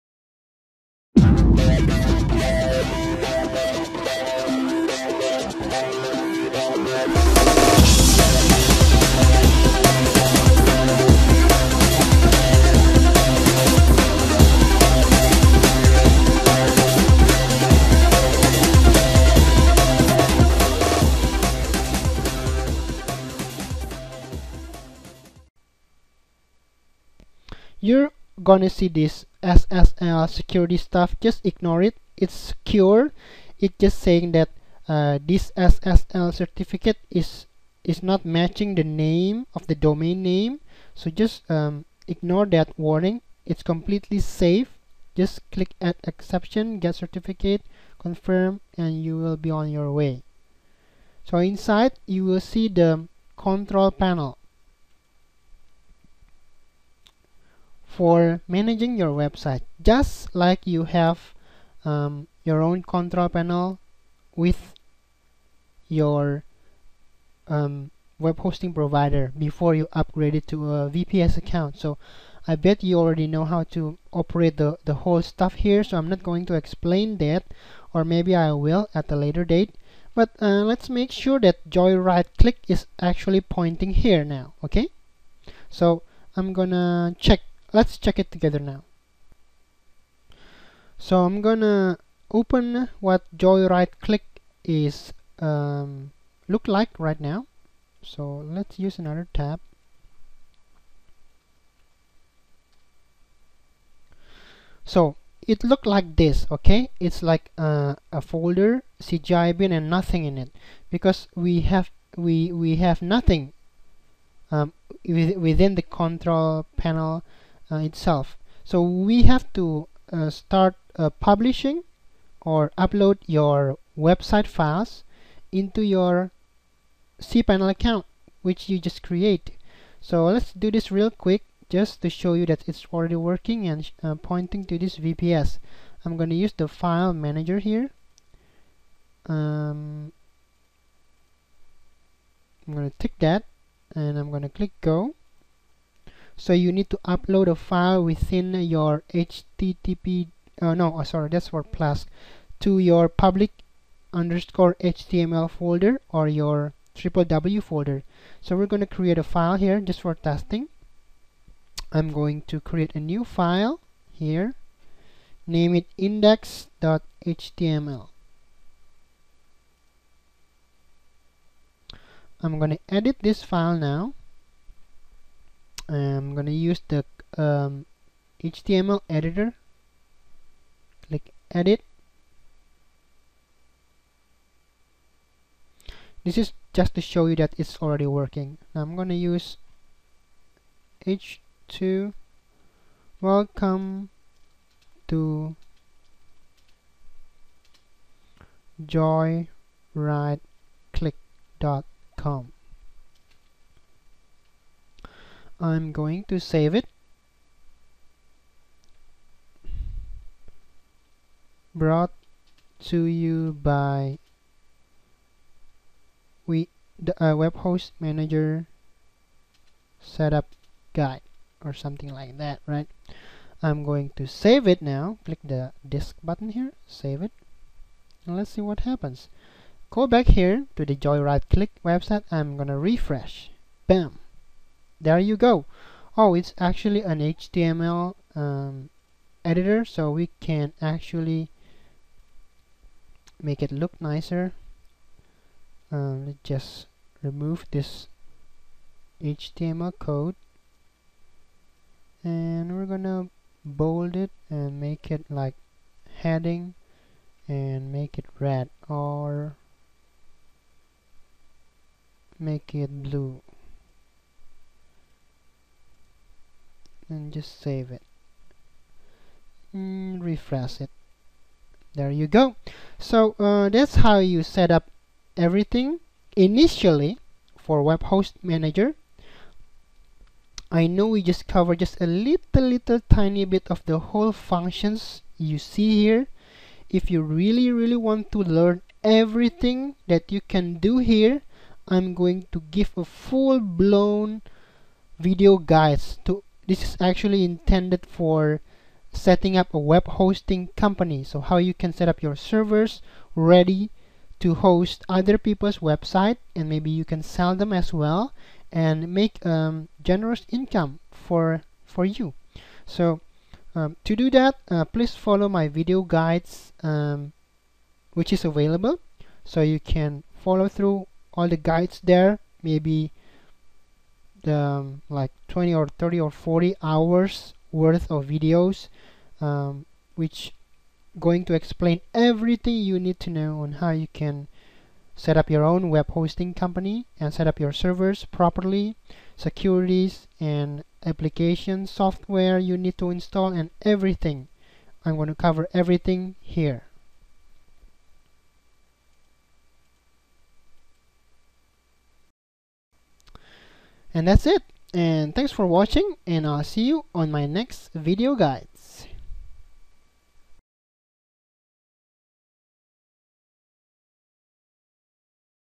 WHM Setup and Configuration (Part 7/7)
Configuring WHM in your VPS to start your hosting business